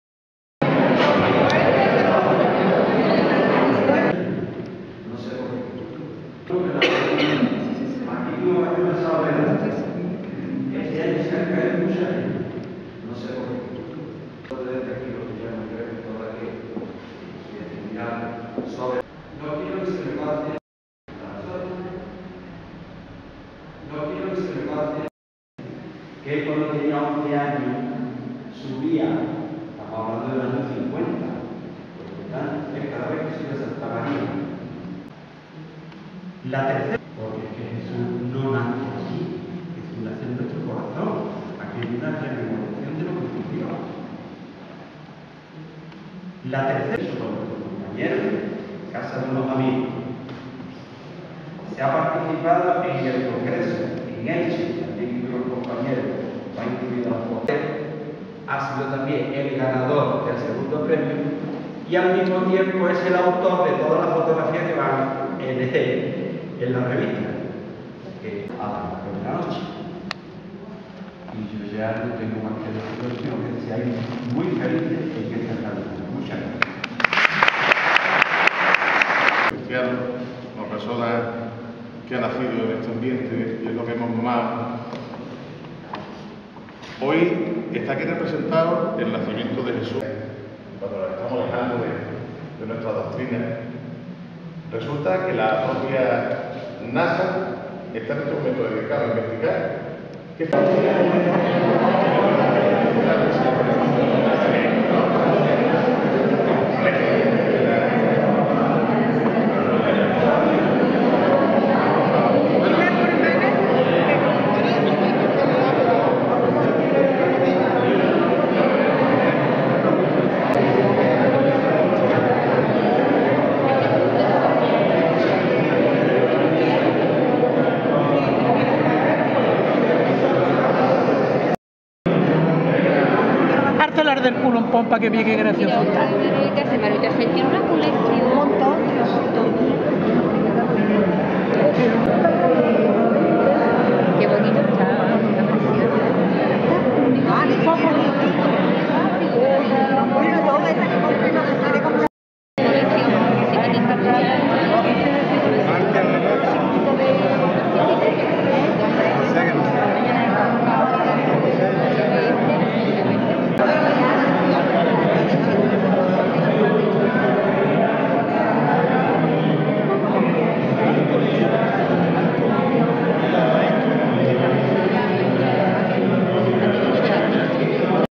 EXPOSICIÓN DE NIÑOS JESÚS EN ARCOS DE LA FRONTERA 2011
EXPOSICIÓN DE  " NIÑO JESÚS " EN SALÓN CULTURAL SAN MIGUEL DE ARCOS, ORGANIZADA POR LA ASOCIACIÓN DE BELENISTA LA ADORACIÓN EN COLABORACIÓN CON EL AYUNTAMIENTO DE ARCOS .